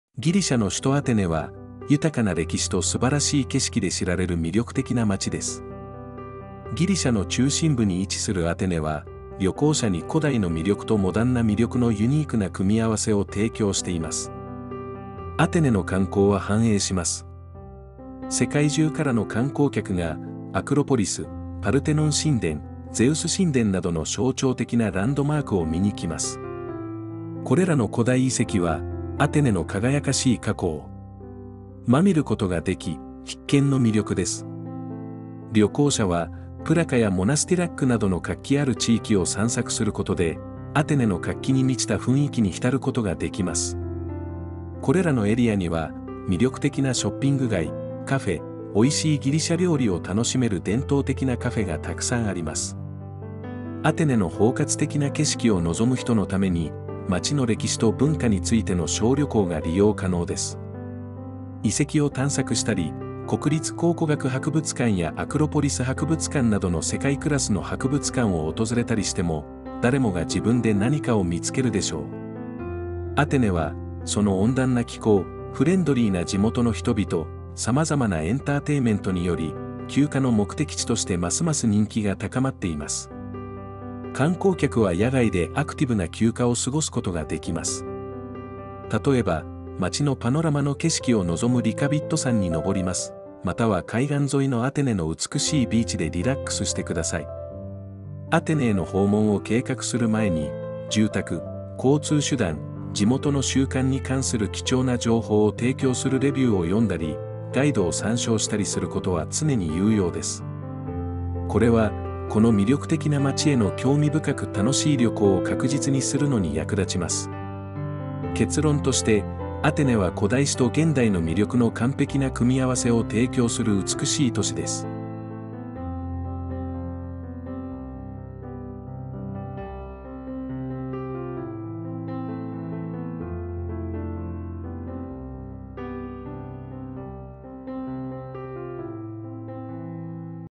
アテネの街を4kで。ギリシャ、アテネの見所
ギリシャの首都アテネは、豊かな歴史と素晴らしい景観で知られる魅力的な都市です。ギリシャの中心に位置するアテネは、古代のアトラクションと現代的な魅力が融合したユニークな魅力を旅行者に提供しています。

アクロポリス、パルテノン神殿、オリンピック・ゼウス神殿など、ギリシャを代表する建造物を一目見ようと世界中から観光客が訪れます。これらの古代遺跡は、アテネの輝かしい過去を垣間見ることができ、必見の観光スポットとなっている。

また、プラカやモナスティラックなどの活気あるエリアを散策すれば、アテネの活気ある雰囲気に浸ることができる。これらのエリアには、魅力的なショッピングストリートやカフェ、おいしいギリシャ料理を楽しめる伝統的なカフェがたくさんある。

アテネを総合的に楽しみたい方には、アテネの歴史や文化に関するエクスカーションもご用意しています。遺跡を探索したり、国立考古学博物館やアクロポリス博物館などの世界的な博物館を訪れたり、誰もが自分自身のために何かを見つけることができます。

アテネは温暖な気候、フレンドリーな地元の人々、バラエティに富んだエンターテイメントで、休暇の目的地として人気が高まっている。例えば、街を一望できるリカヴィト山に登ったり、海岸沿いの美しいビーチでリラックスしたりと、アクティブな休暇を過ごすことができます。

アテネ訪問を計画する前に、住宅、交通手段、地元の習慣などに関する貴重な情報を提供してくれるガイドを参考にしたり、レビューを読んだりすることは常に有益です。そうすることで、この魅力的な都市での興味深く楽しい旅を確実にすることができる。

結論として、アテネは古代の歴史と現代の魅力が見事に融合した美しい都市である。